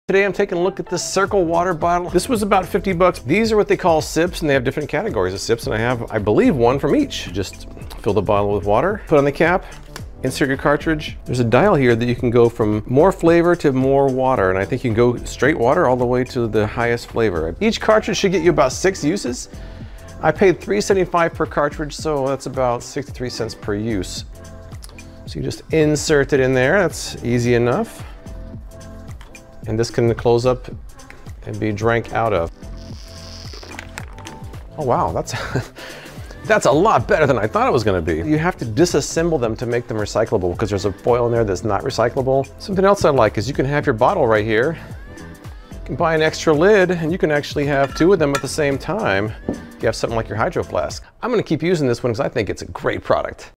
Trying the Cirkul Water Bottle
Testing the Cirkul, which is a water bottle that uses flavor pods. I just happen to like it.